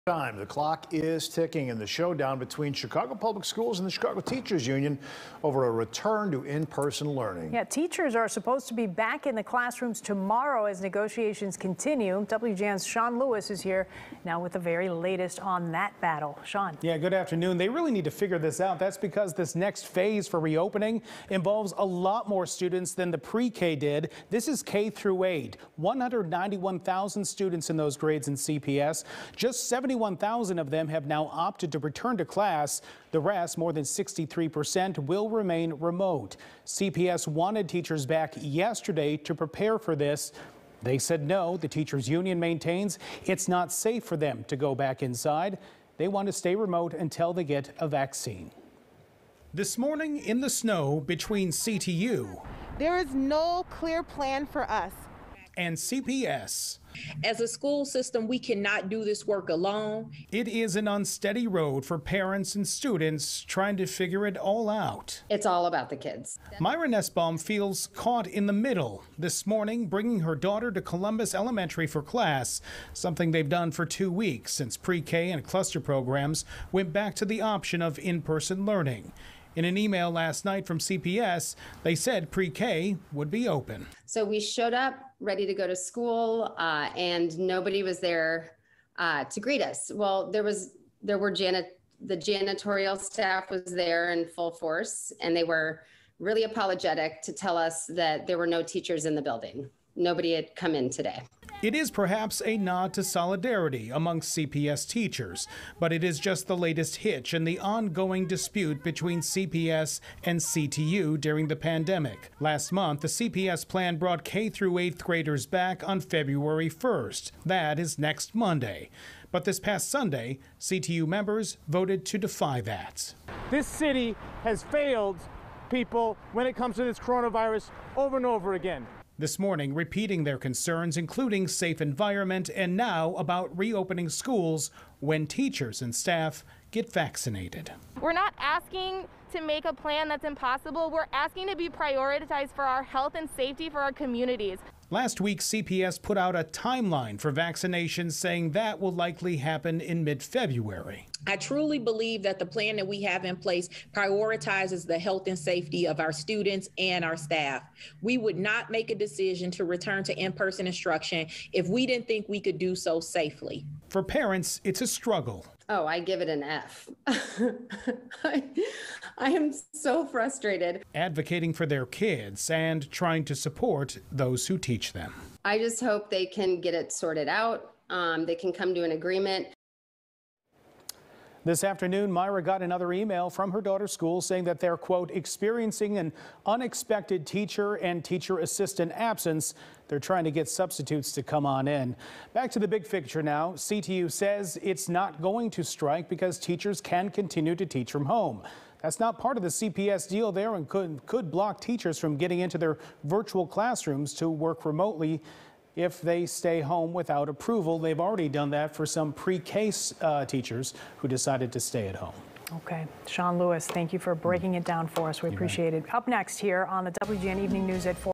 Battle between CPS and CTU continues as teachers ordered back to classrooms starting Wednesday
There's than a week until 71,000 more Chicago Public School students are scheduled to return to the classroom -- but the Chicago Teachers Union says the district's plan is not holding up.